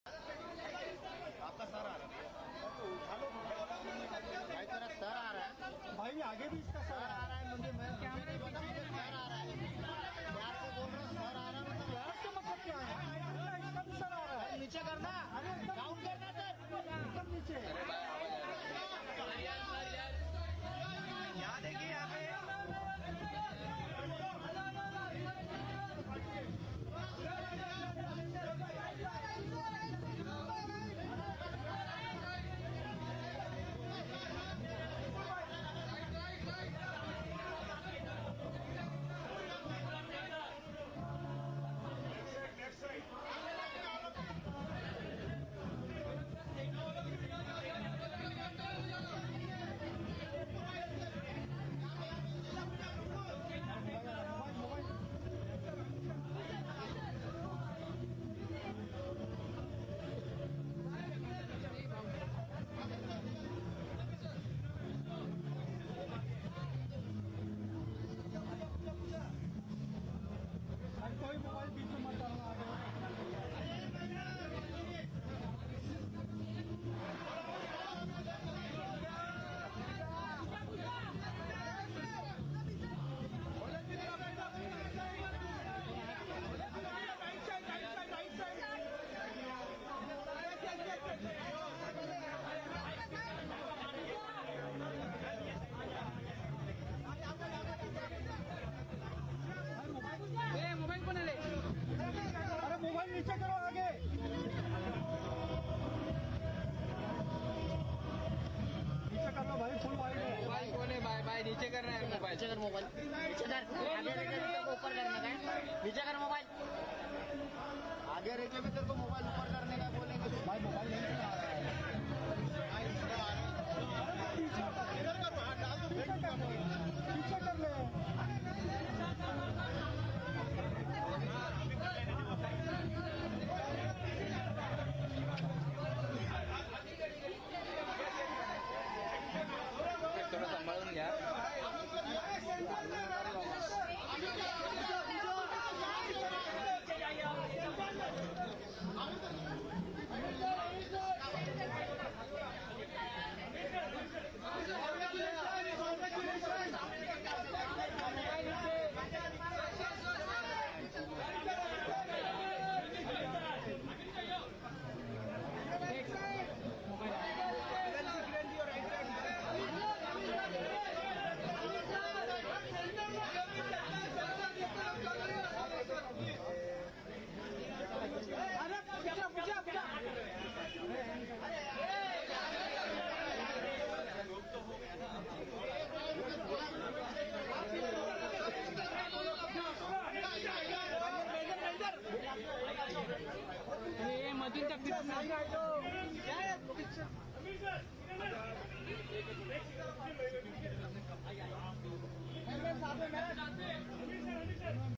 Ira Khan Wedding : नुपुर शिखरे की हुईं आयरा खान, फैमिली और करीबी दोस्तों की मौजूदगी में हुई शादी
IraKhanWedding : नुपुर शिखरे की हुईं आयरा खान, अभिनेता आमिर खान की बेटी इरा खान और नुपुर शिखरे ने परिवार और करीबी दोस्तों की मौजूदगी में शादी कर ली है। दोनों की शादी का रिसेप्शन मुबंई के ताज लैंड्स एंड हो रहा है।